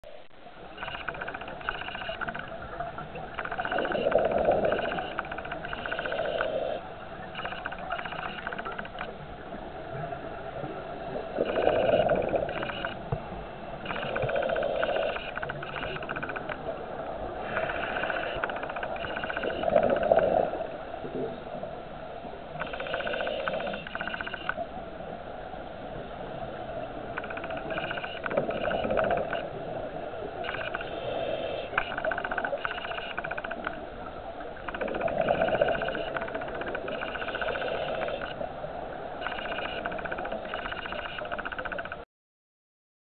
Shipwreck Avantis III (4)
Diving the shipwreck of Avantis III on Agkistri Island (Dorousa)